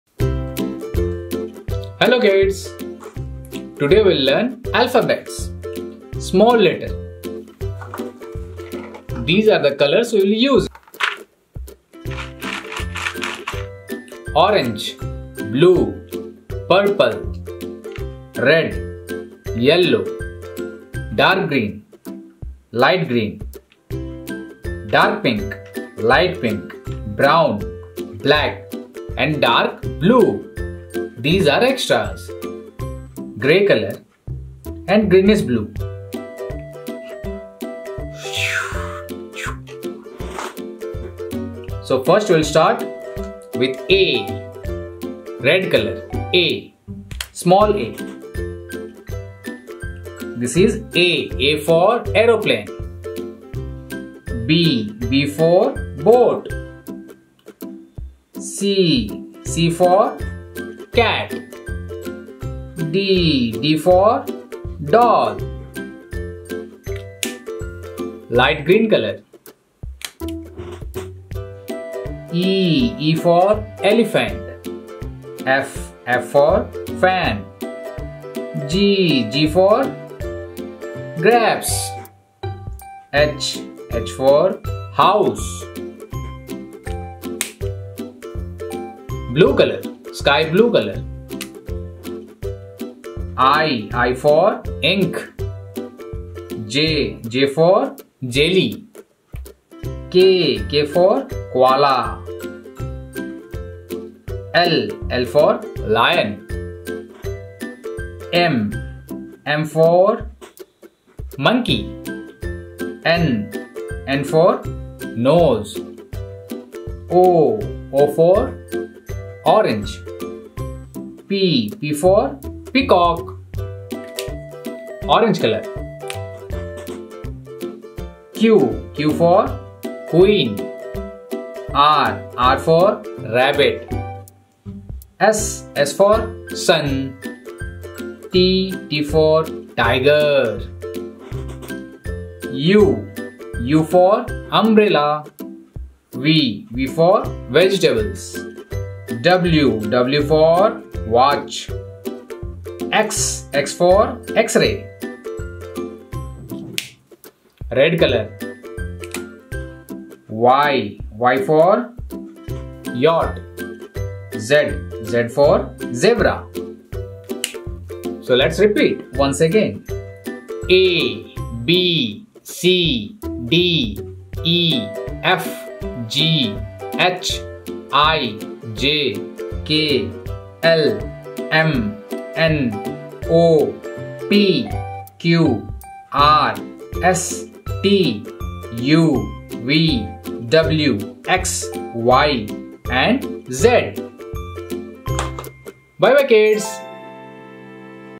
Learn to read and write Alphabets lowercase, small letters abc | A for apple, b for ball | abcd
Hello Kids, In this video, you will learn to read and write Abc alphabets in lowercase or small letters. Trace the small letters alphabets from A to Z.